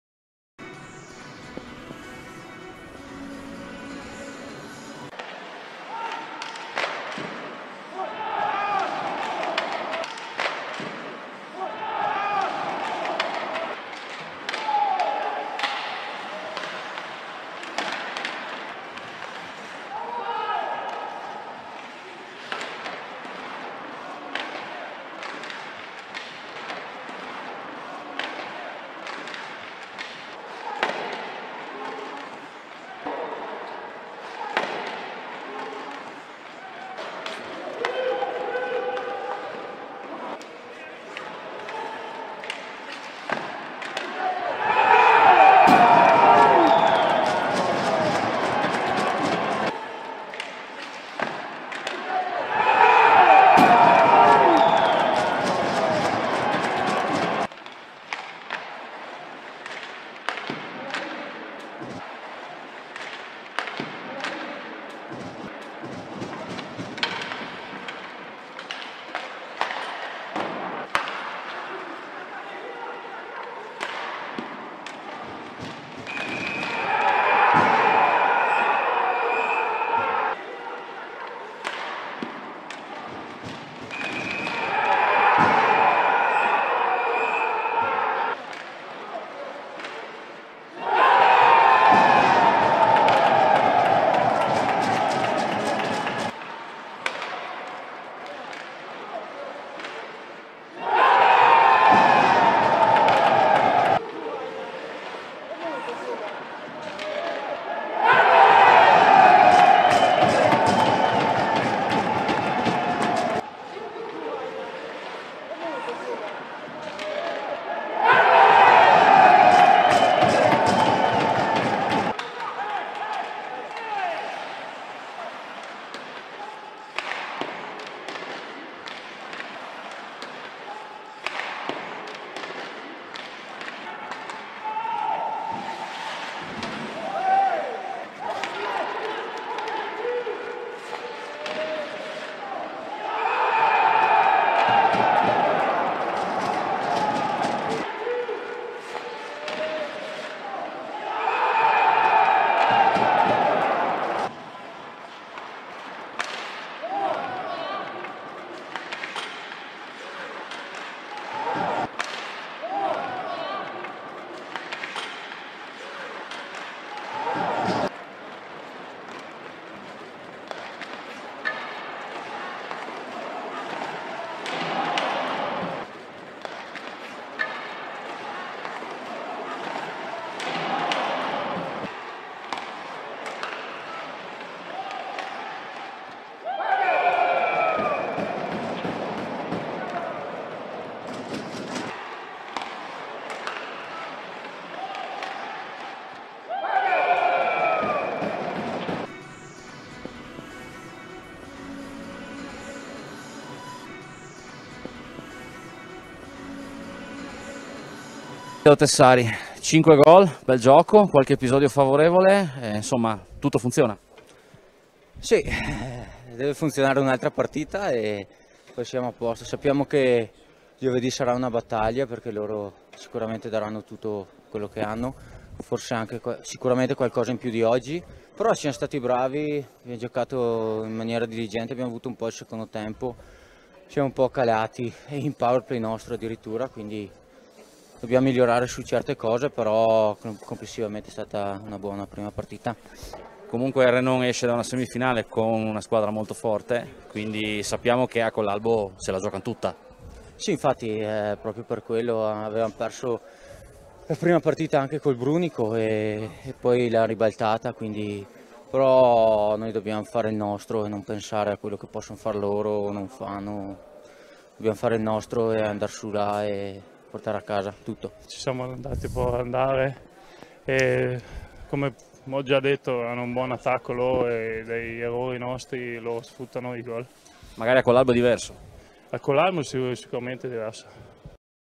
Highlights ASIAGO HOCKEY 1935 vs RITTNER BUAM 09/02/2021 (5:0)
Gara 1 Finale Scudetto SERIE A 2020/2021